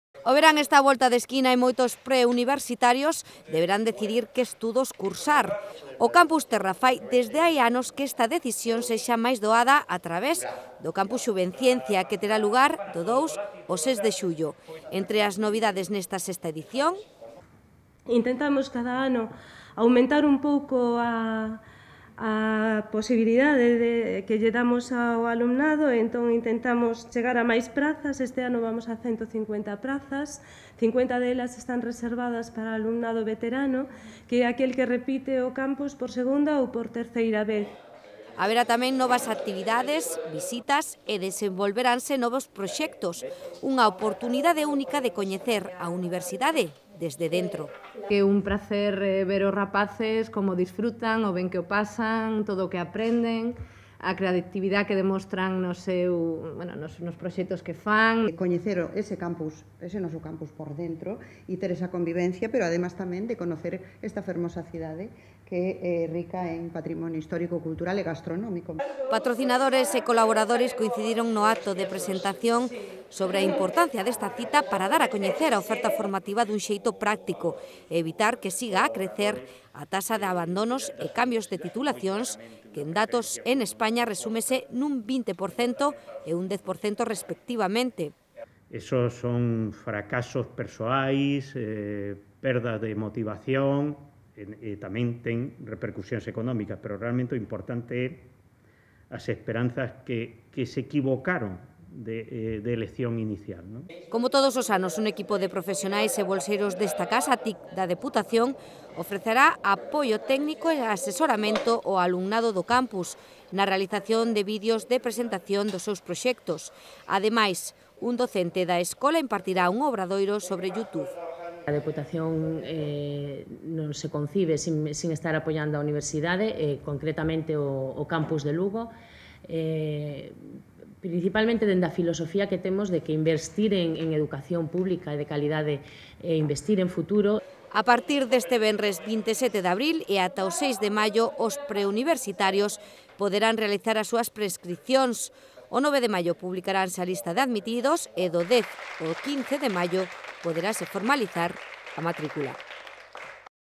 Presentación Xuvenciencia 2018
O verán está a volta da esquina e moitos pre-universitarios deberán decidir que estudos cursar. O Campus Terra fai dende hai anos que esta decisión sexa máis doada a través do Campus Xuvenciencia que terá lugar do 2 ao 6 de xullo. Entre as novidades nesta VI Edición haberá novas actividades, visitas e desenvolveranse novos proxectos, unha oportunidade única de coñecer a universidade desde dentro. Patrocinadores e colaboradores coincidiron no acto de presentación sobre a importancia desta cita para dar a coñecer a oferta formativa dun xeito práctico e evitar que siga a crecer a taxa de abandonos e cambios de titulacións que en datos en España en un 20% e un 10% respectivamente. Como todos os anos un equipo de profesionais e bolseiros da Tic da Deputación ofrecerá apoio técnico e asesoramento ao alumnado do Campus